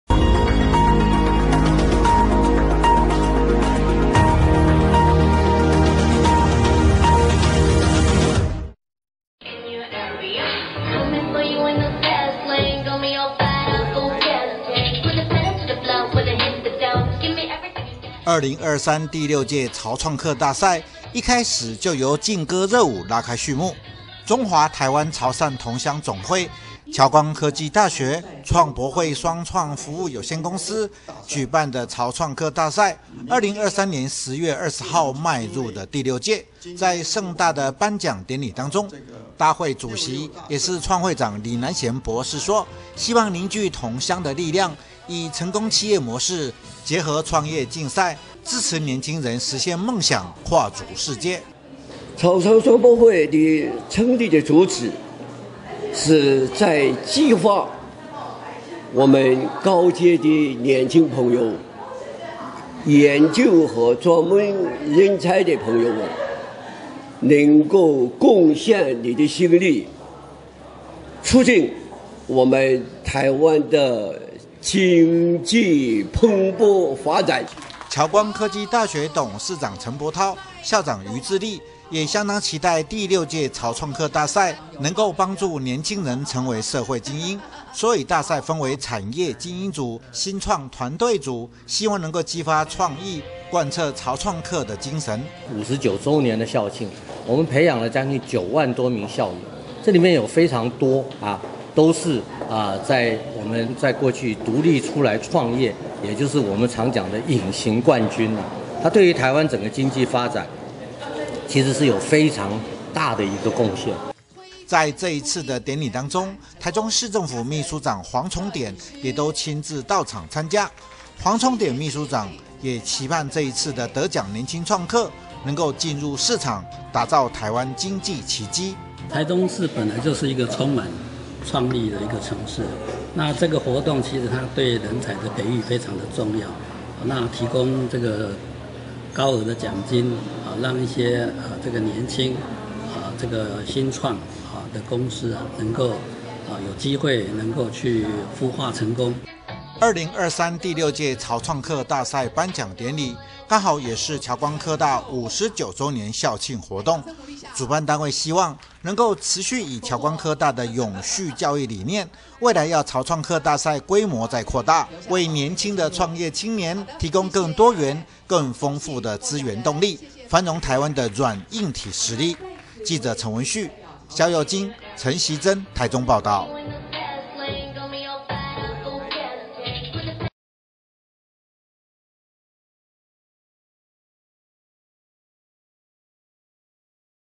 《台傳媒新聞》2023第六屆潮創客大賽 10/20僑光科大熱烈登場 20231023
【記者 陳文旭、蕭又菁、陳習珍 / 台中報導】
2023第六屆「潮創客大賽」，一開始就由勁歌熱舞拉開序幕，中華台灣潮汕同鄉總會、僑光科技大學、創博匯雙創服務有限公司舉辦的潮創客大賽，2023年10月20號邁入了第六屆，在盛大的頒獎典禮當中，大會主席也是創會長李南賢博士說，希望凝聚同鄉的力量，以成功企業模式結合創業競賽，支持年輕人實現夢想跨足世界。
僑光科技大學董事長陳伯濤、校長余致力也相當期待第六屆潮創客大賽能夠幫助年輕人成為社會精英，所以大賽分為產業菁英組、新創團隊組，希望能夠激發創意，貫徹潮創客的精神。
在這次的典禮當中，台中市政府秘書長黃崇典也都親自到場參加，黃崇典秘書長也期盼這次的得獎年輕創客，能夠進入市場打造台灣經濟奇蹟。
2023第六屆潮創客大賽頒獎典禮，剛好也是僑光科大59週年校慶活動，主辦單位希望能夠持續以僑光科大的永續教育理念，未來要潮創客大賽規模再擴大，為年輕的創業青年提供更多元、更豐富的資源動力，繁榮台灣的軟硬體實力。